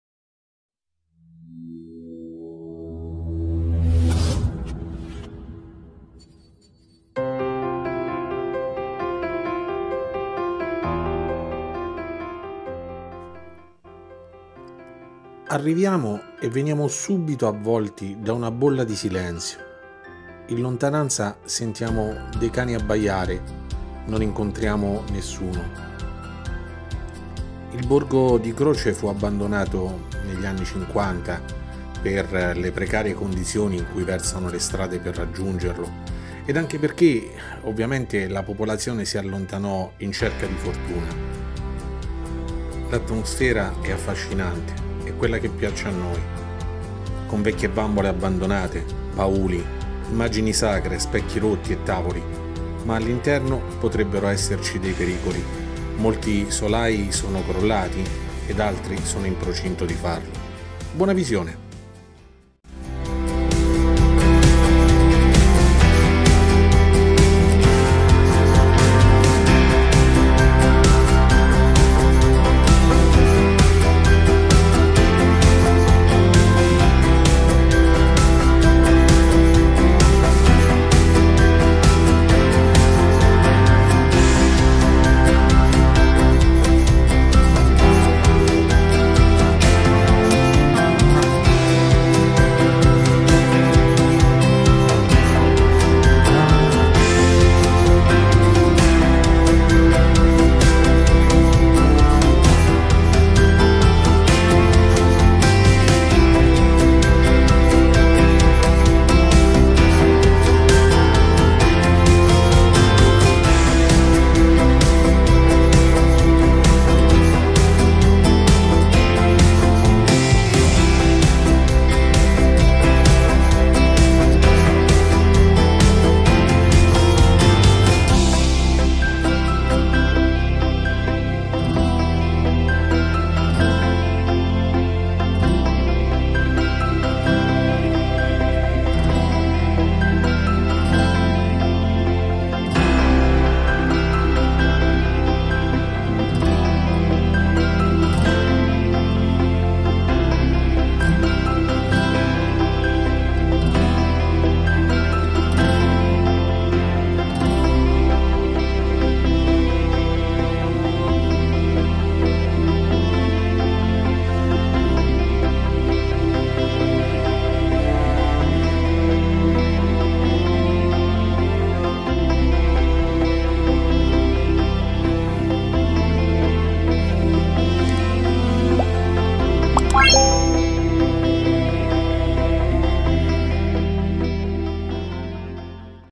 Croce di Croce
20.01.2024

Il borgo di Croce si trova ai piedi del monte Maggiore, una volta era una tappa dei devoti che in pellegrinaggio arrivavano fino ala chiesta di San Salvatore, a strapiombo sul monte

Croce si trova inerpicato su un monte ed è stato un centro abitato fondato a causa delle incursioni saracene più di mille anni fa. Croce fa parte del Comune di Rocchetta santa Croce, un comune diffuso con meno di 400 abitanti, tutti concentrati nella frazione di Rocchetta.

Sia Croce che Rocchetta nacquero intorno al 900 d.c dall’abbandono, a causa delle incursioni saracene, dell’antica Cales che si trovava appena a valle dei promontori su cui sorgono i due centri;